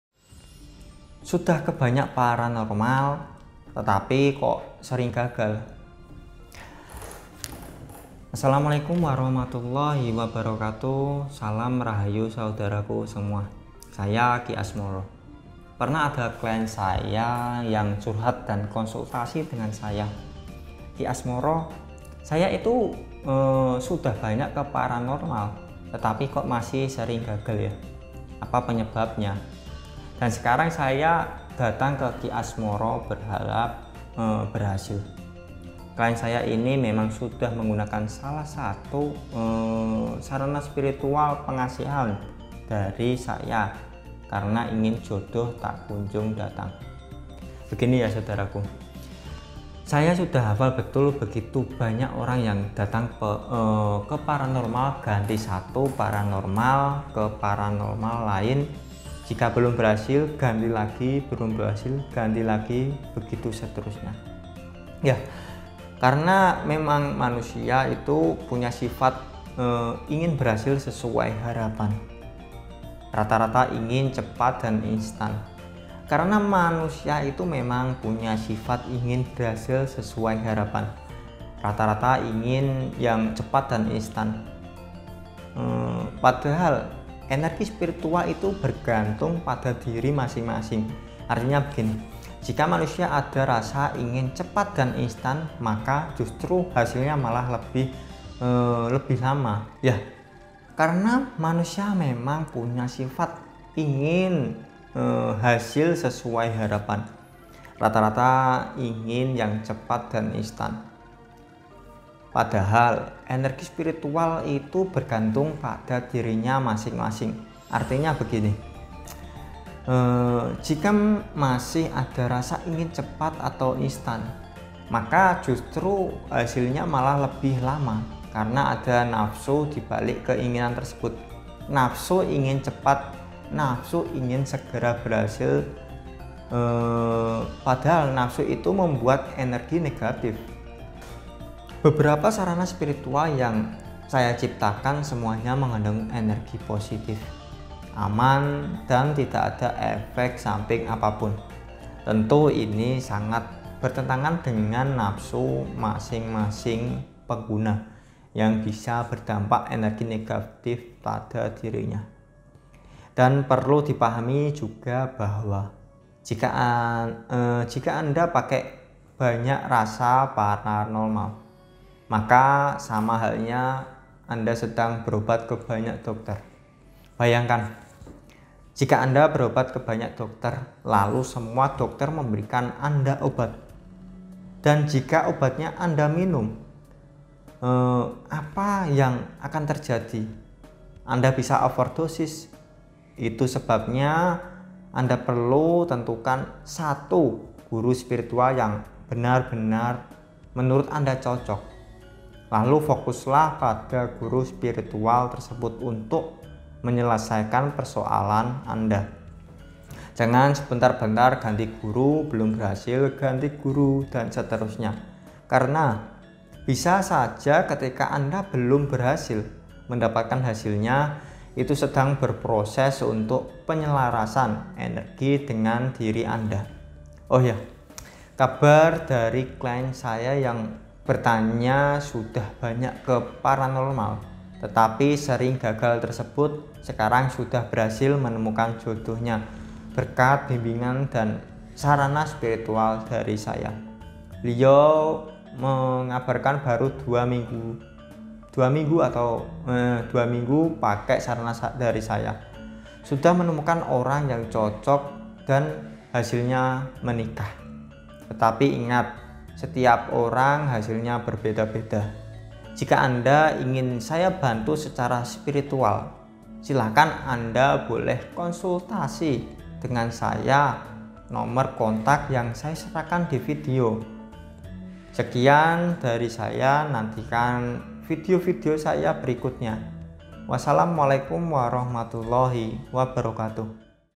SUDAH BANYAK KONSULTASI DENGAN PARANORMAL TAPI KOK MASIH GAGAL, APA PENYEBABNYA?
Untuk konsultasi layanan atau pemesanan produk, silahkan bisa langsung hubungi staf kami, Mbak Sinta di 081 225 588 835 atau Anda bisa langsung klik link ini